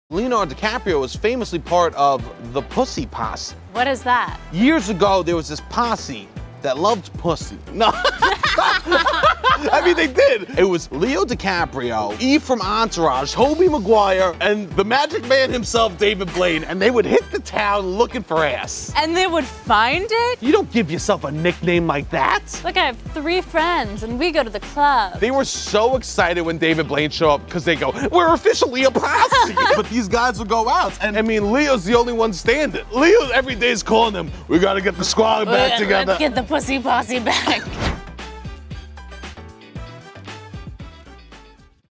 Who remembers Leonardo DiCaprio's old posse? | Stoopin' New York Post
Years ago, Leonardo DiCaprio along with pals Tobey Maguire, Kevin Conolly, and David Blaine were all part of a group of partiers dubbed the Pussy Posse.